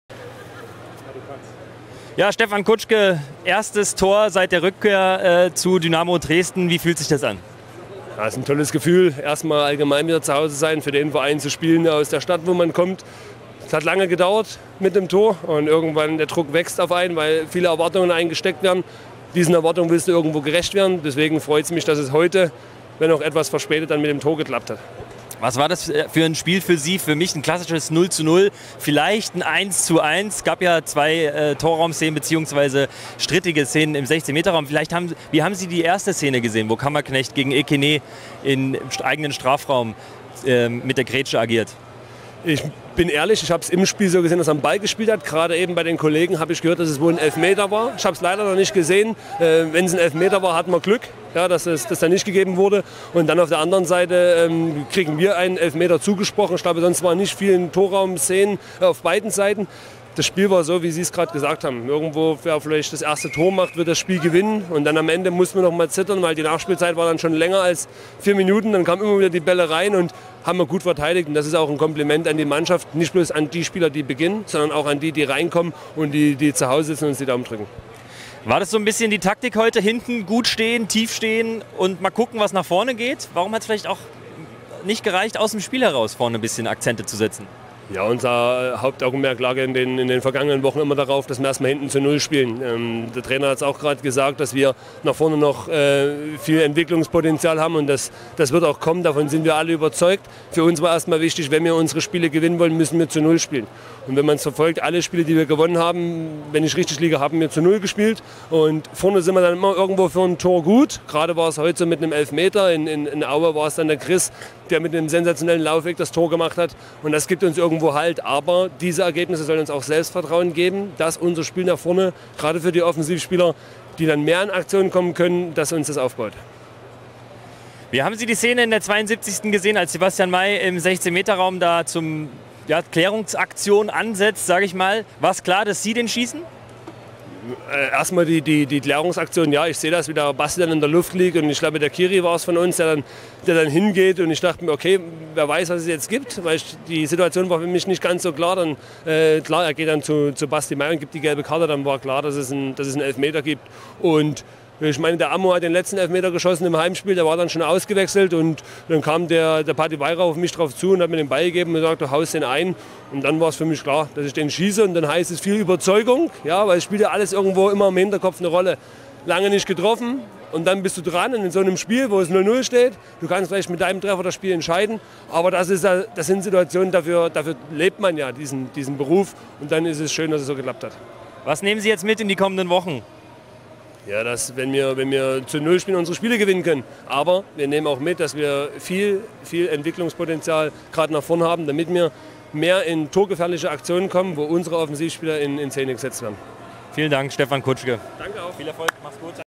SG Dynamo Dresden | Stefan Kutschke: "Viel Entwicklungspotenzial nach vorn" | Sport im Osten | MDR
Nach seinem ersten Tor nach der Rückkehr nach Dresden blickte Stefan Kutsche glücklich auf das Spiel gegen Duisburg zurück. Offensiv sieht er Entwicklungsmöglichkeiten.